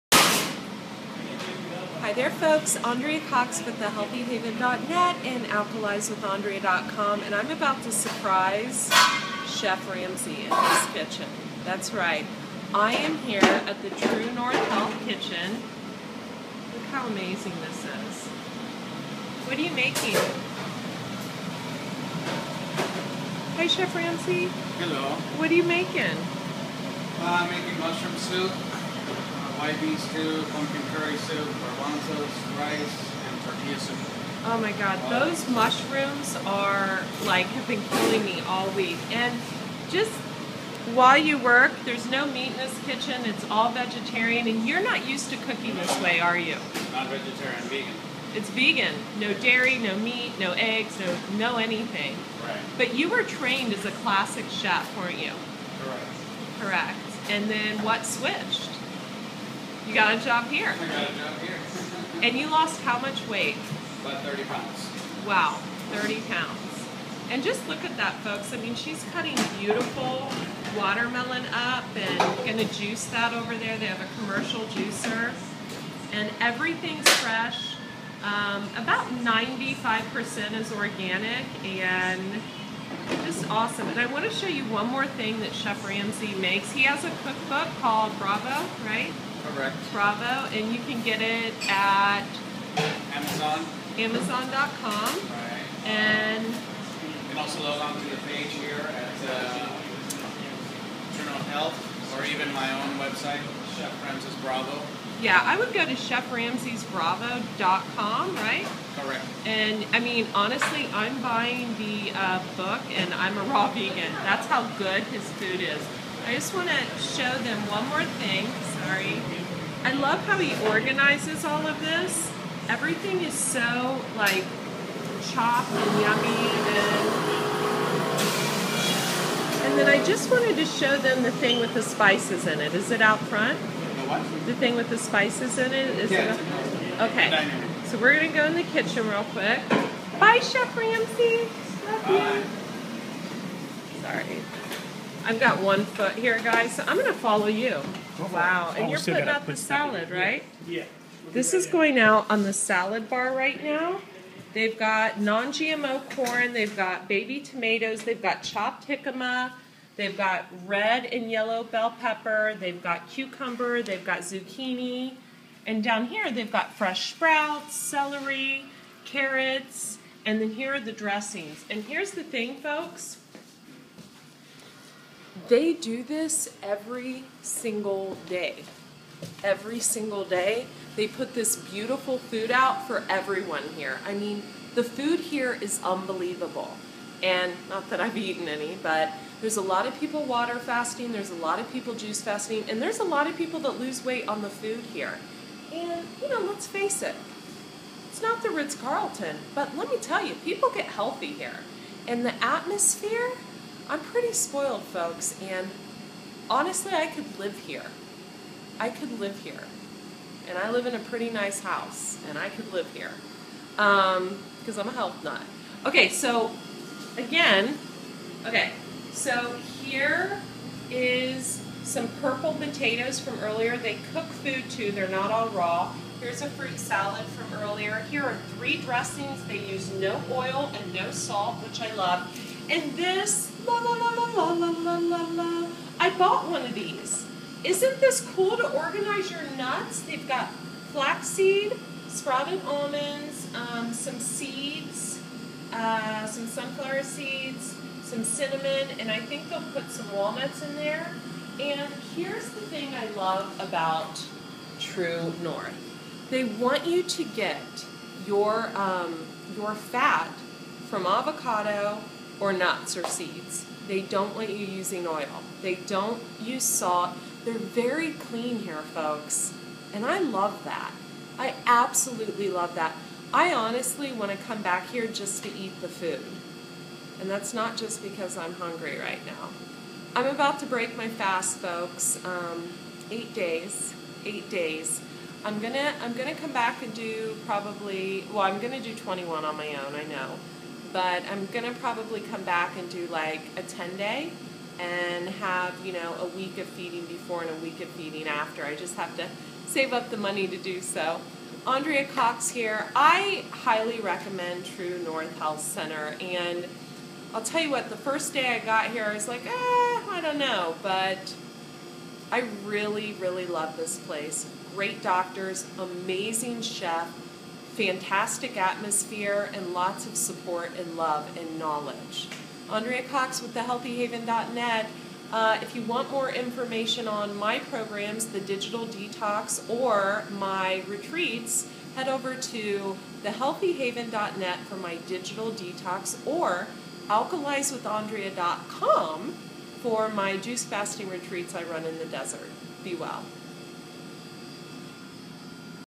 Day 7 of my water fast! Meet Chef Ramses Author of Bravo at True North!
Hello Facebook, You Tube and Twitter family! I shot this video at the end of May at True North Health Center! I was on day 7 of my water fast (I think). It was such a beautiful time and several people go there just to eat! I am returning here in just a few months to under go a longer water fast and wanted to share with you the BEAUTIFUL food that they make here. Enjoy and don't forget to check out my Digital Detox program located under "programs" found at this
Namaste